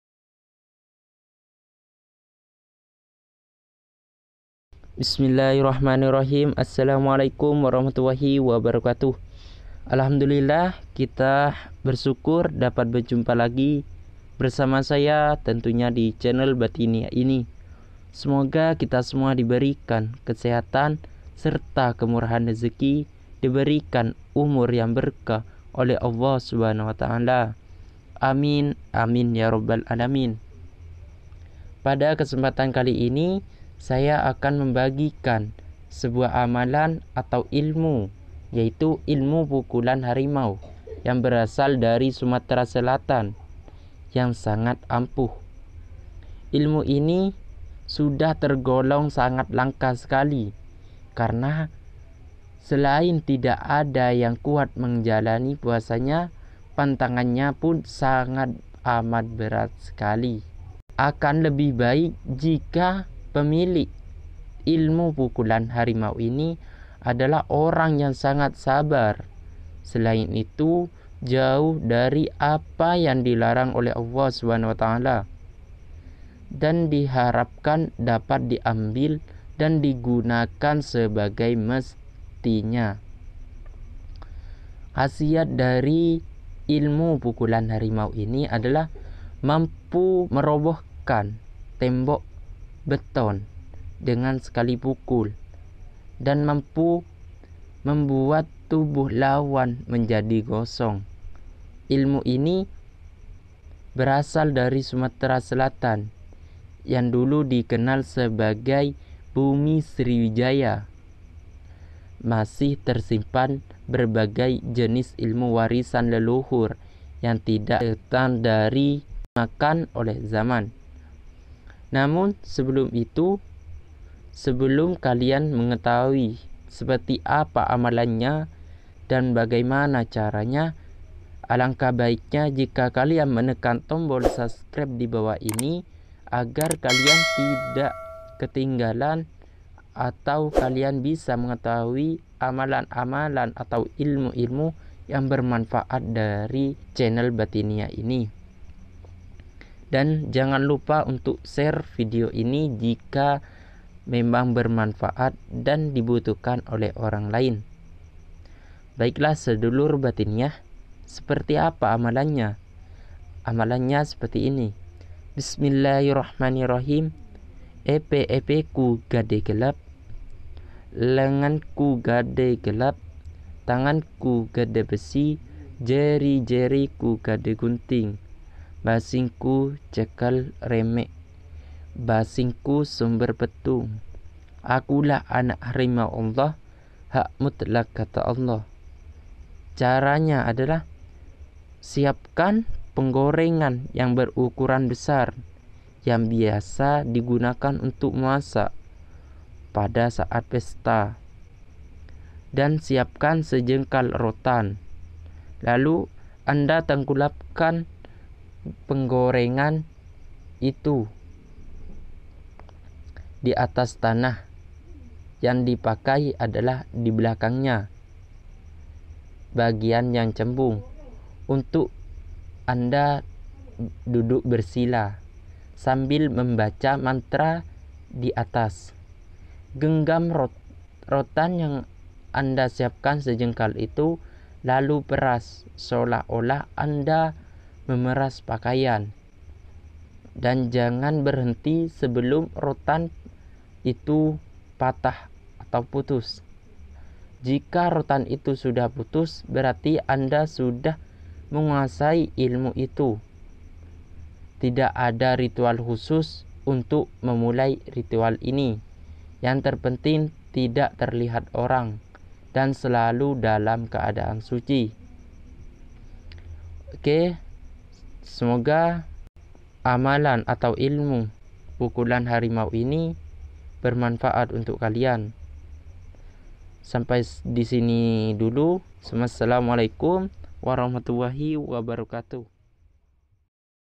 Amalan pukulan harimau allah //doa harimau allah || Batiniyah
amalan pukulan harimau allah.
khasiat dari pukulan ini bisa merobohkan beton yang sangat kuat. ilmu ini berasal dari sumatra selatan.

di wik wik wik pocong